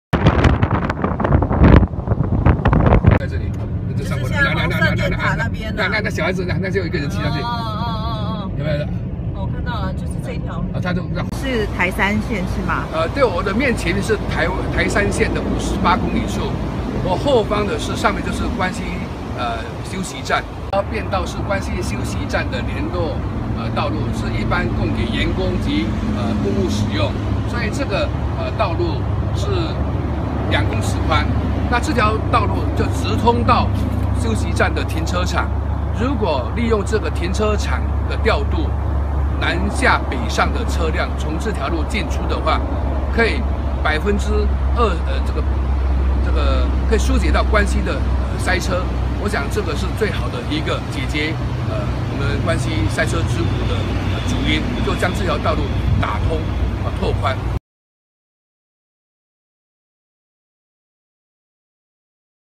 救關西塞車 鎮代會爭取：拓寬、開放服務區公務便道
新竹縣關西鎮近年假日飽受國道3號車流湧入的塞車之苦，鎮代會爭取拓寬國道關西服務區西南側現有公務便道，作為關西交流道的東側聯外匝道，限制僅供南來北往的自小客車行走，直接打通國道3號和省道台3線，以解決假日塞車困境，代表會主席劉德樑說，近日就將具文向縣府陳情，請求進行可行性評估。